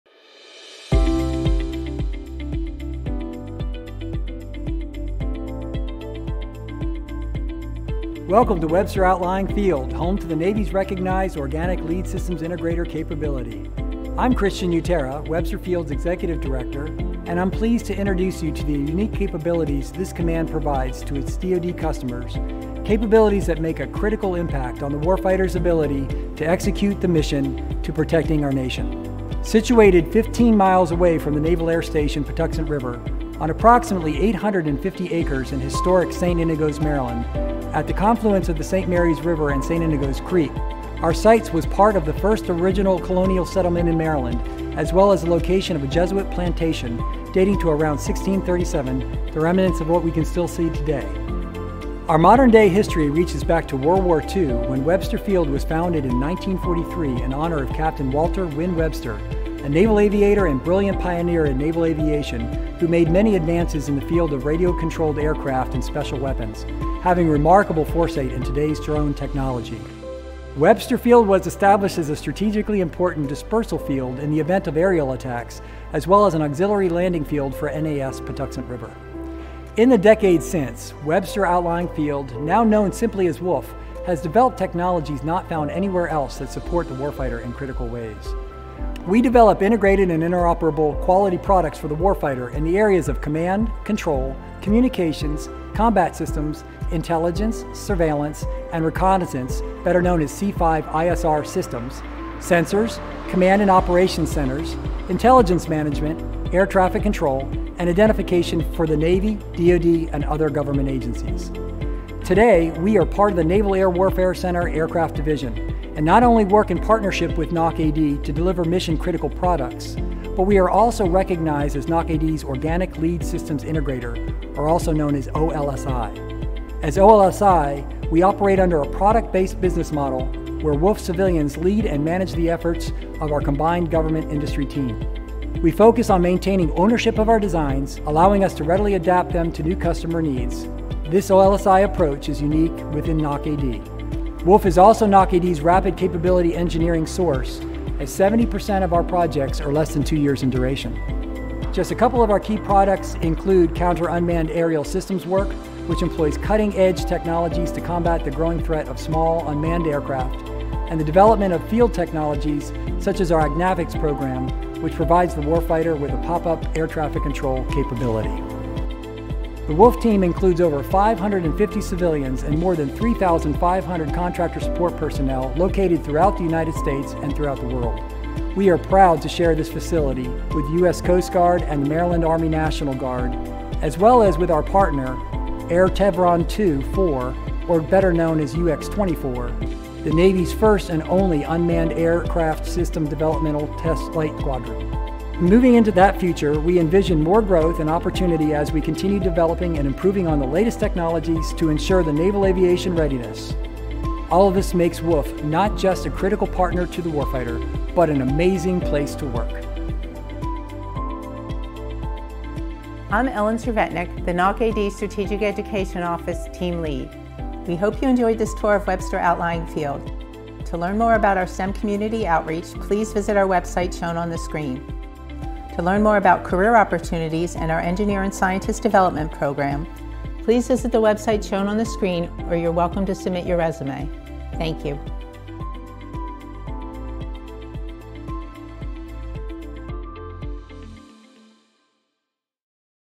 Welcome to NAWCAD's site at Webster Outlying Field!
Welcome to NAWCAD's Webster Outlying Field, also known as WOLF—a nexus of innovation in St. Inigoes, Maryland. As the Navy's recognized organic Lead Systems Integrator (oLSI), WOLF plays a critical role in safeguarding our skies as well as advancing unmanned flight through our pioneering UX-24 test squadron. Here, we engineer cutting-edge technology, from command control to intelligence surveillance, supporting Navy operations and the Department of Defense.

With over 3,000 dedicated individuals, including civilians, military, and industry partners, we're crafting solutions that tackle today's defense and aerospace challenges head-on. As an organic oLSI, we bridge government and industry expertise to pave the way for a safer, more technologically advanced future. WOLF is one of four of NAWCAD's geolocations most closely situated to command headquarters at Naval Air Station Patuxent River in Southern Maryland.